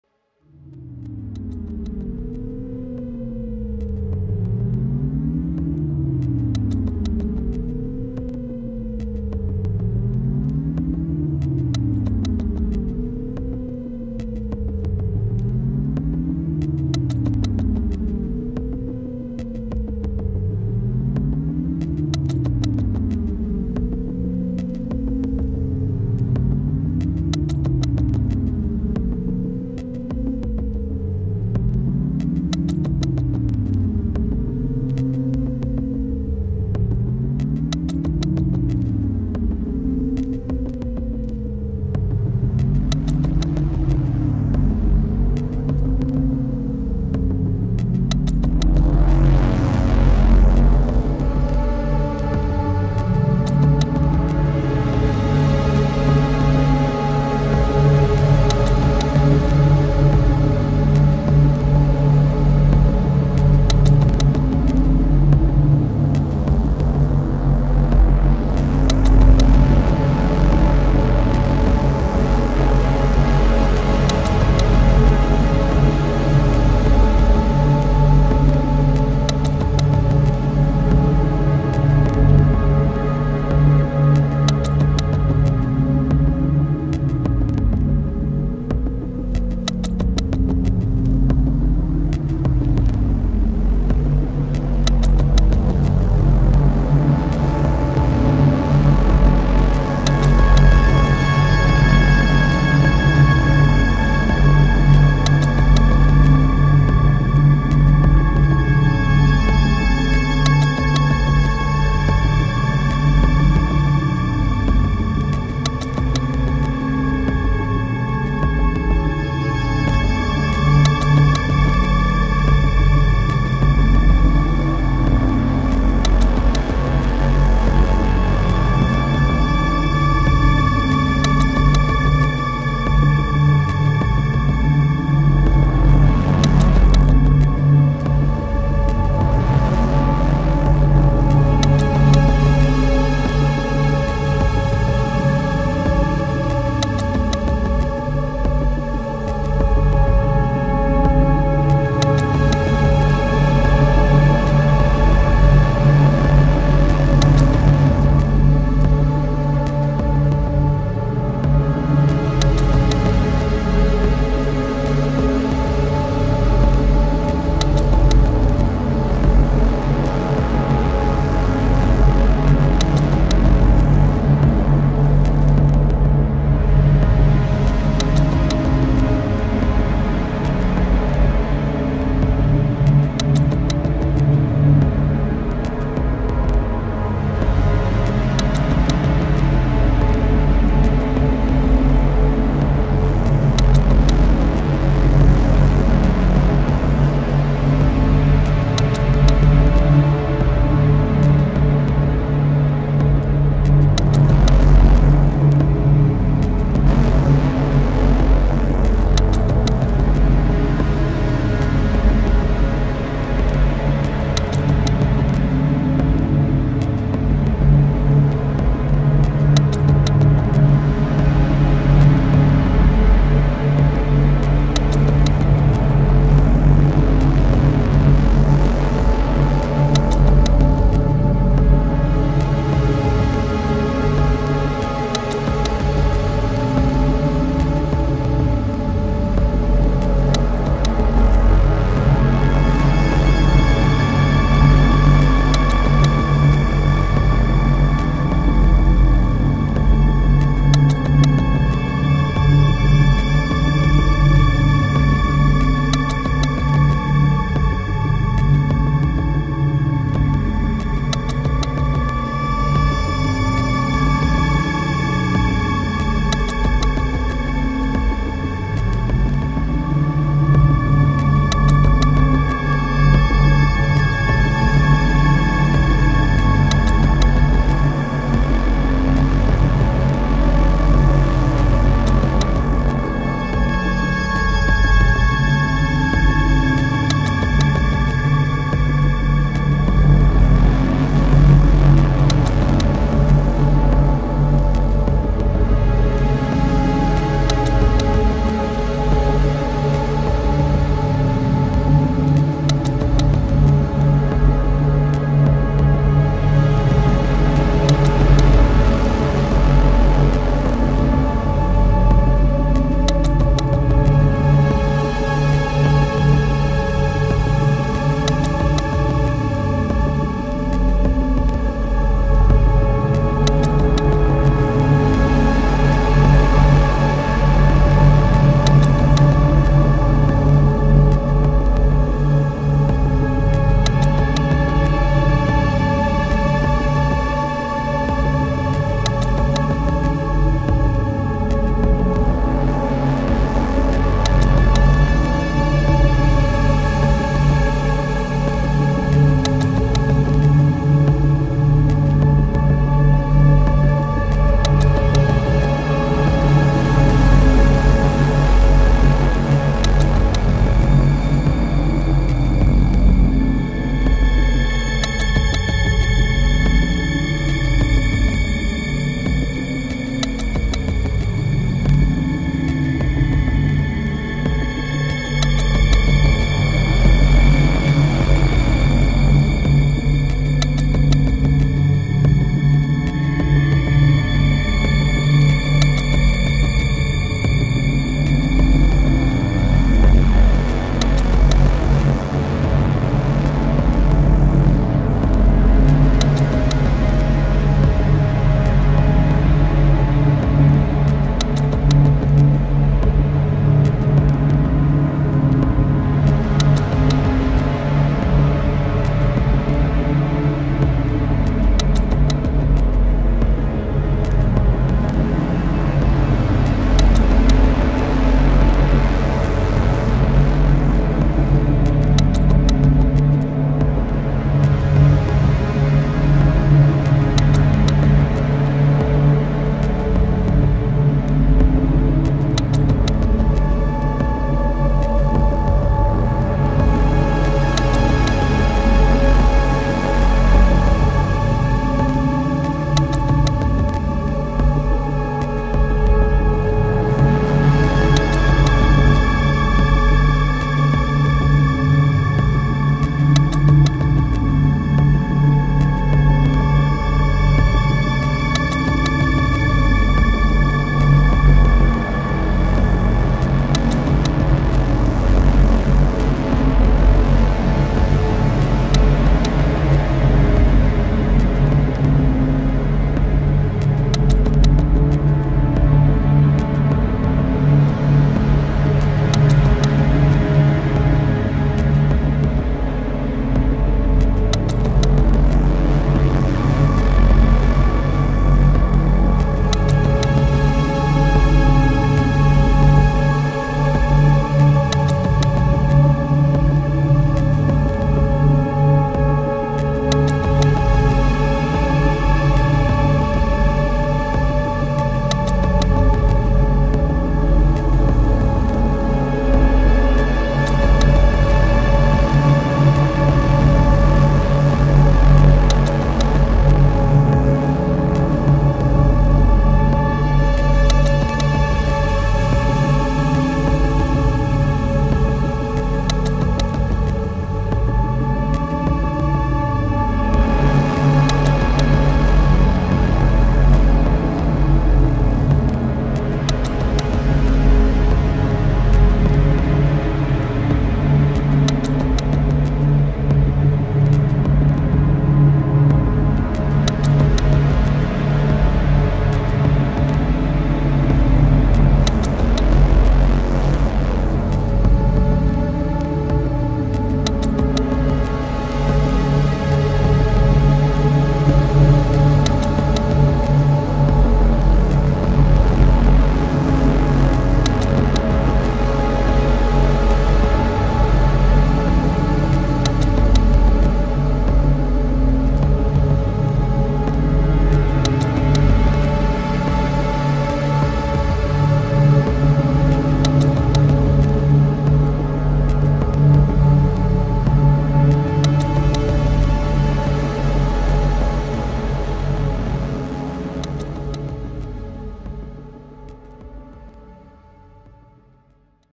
SonicLab Fundamental VST Plugin // Nostalgie Sweep
First use of the new Hainbach inspired VST, Fundamental. Performed live in Usine Hollyhock. One drum track, 3 instances of Fundamental and one instance of Arturia's Buchla Easel.